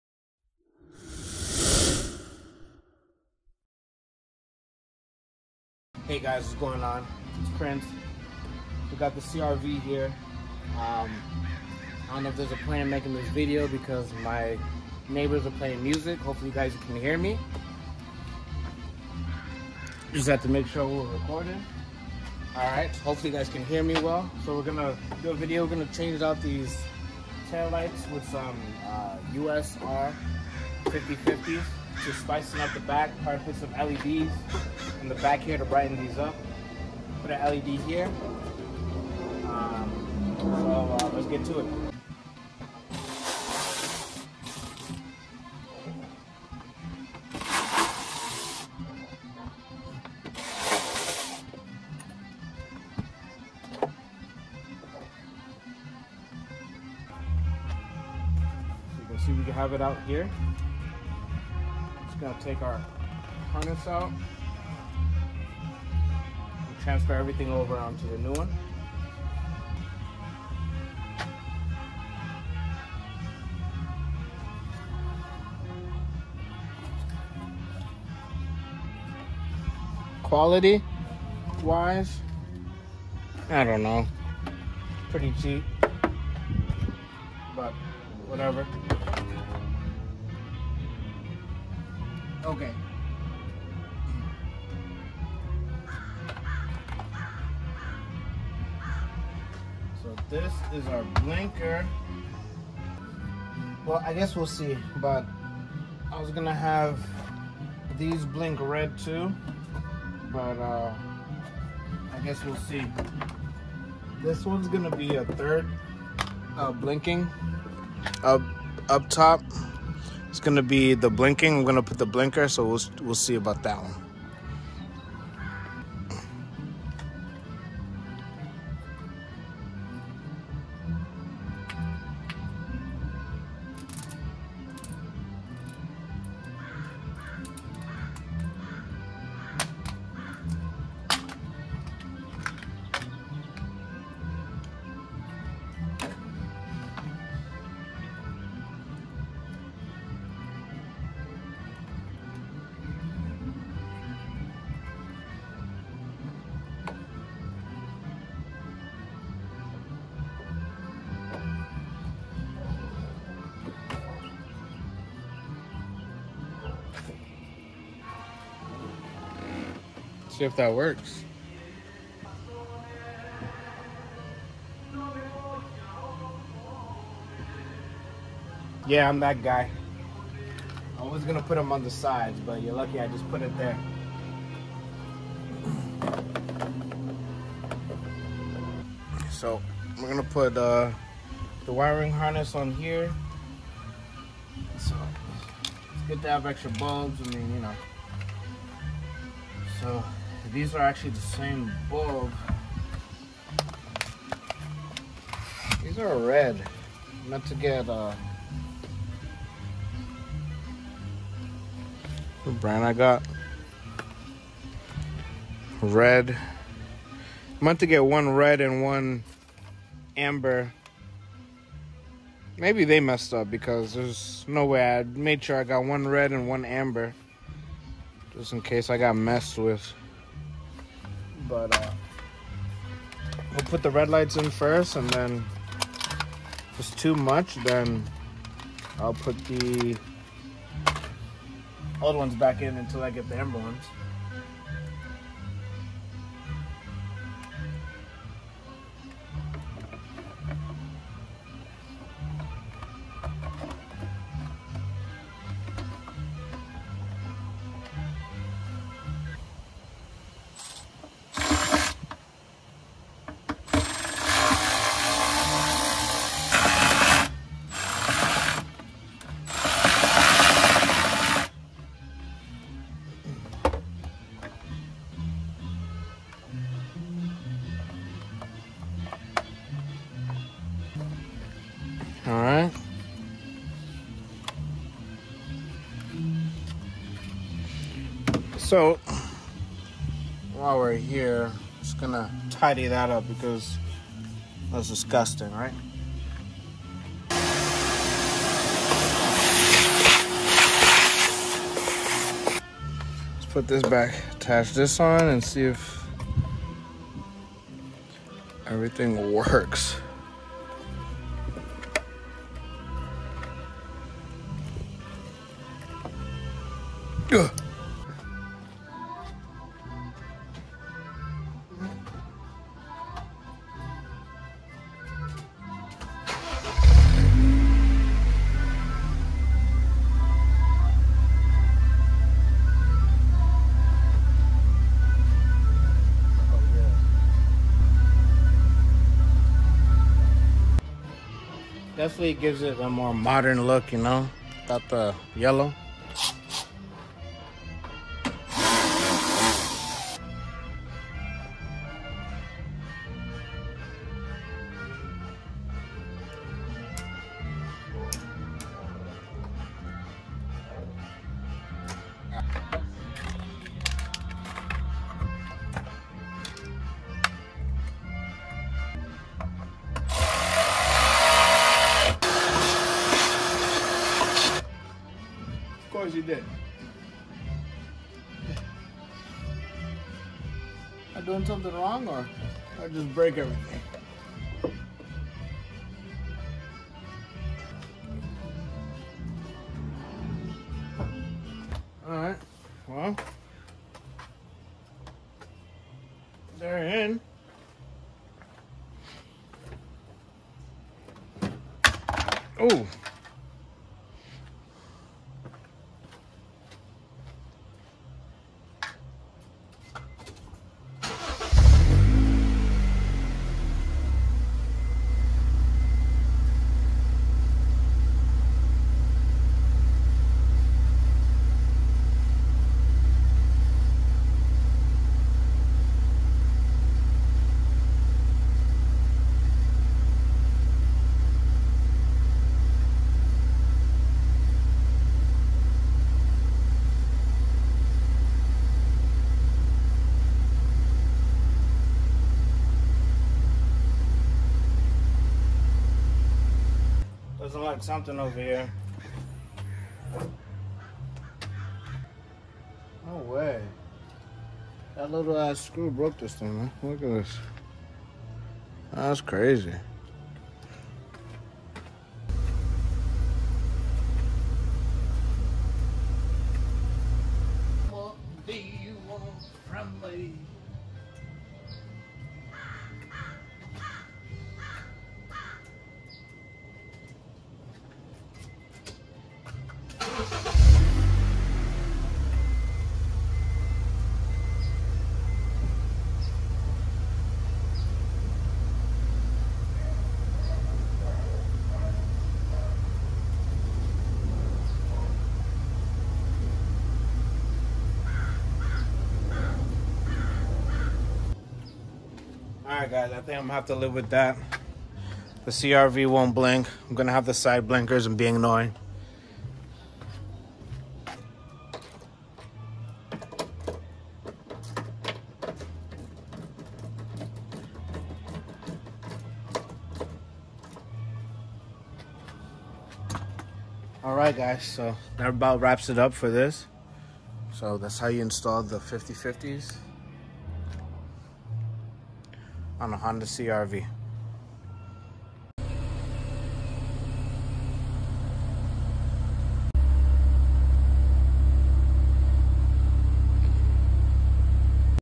97-00 Honda CRV RD1 50/50 Tail lights
Replacing the tail lights on Honda CRV USR 50/50 JDM style tail lights

Sorry about the background music. Neighbors where having fun lol! More CRV content coming soon!

USR 50/50 tail lights

Red Leds

Blinking brakes

Chuchero the world!
God Bless!
Prince 👑